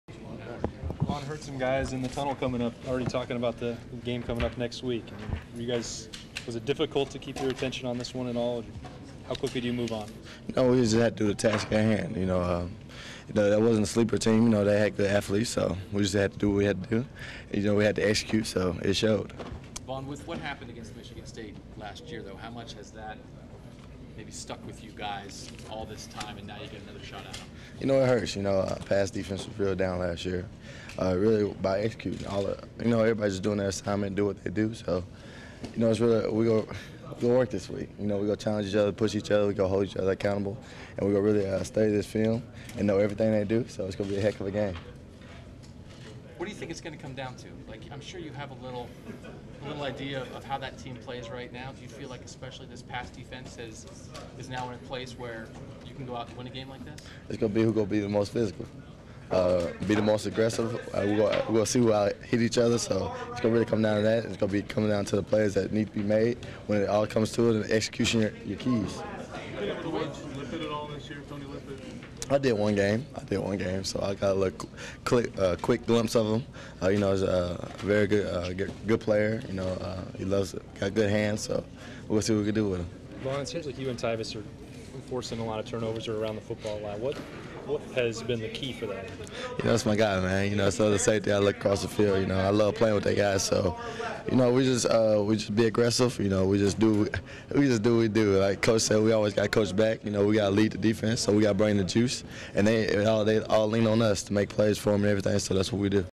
Ohio State So. Safety Vonn Bell 11/1/2014
Buckeyes Safety Vonn Bell after the OSU's 55-14 win over Illinois